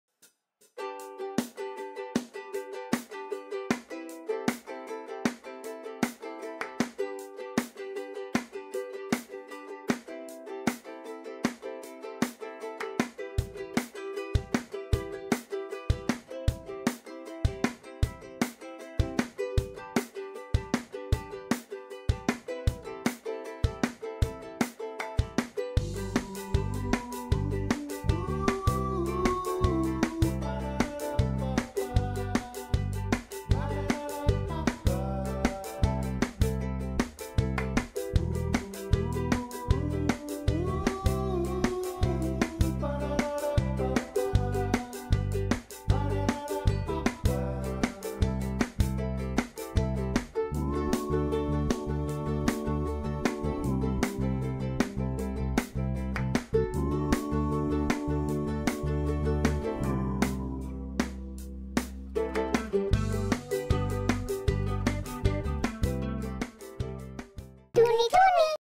Atún Tuny : Pasta corta con ensalada de atún Tuny * RECETA FÁCIL*
Esta exquisita Pasta corta con ensalada de atún Tuny será tu salvación. No sólo es fácil de preparar, también es rápida y perfecta para compartir. ¡Pruébala!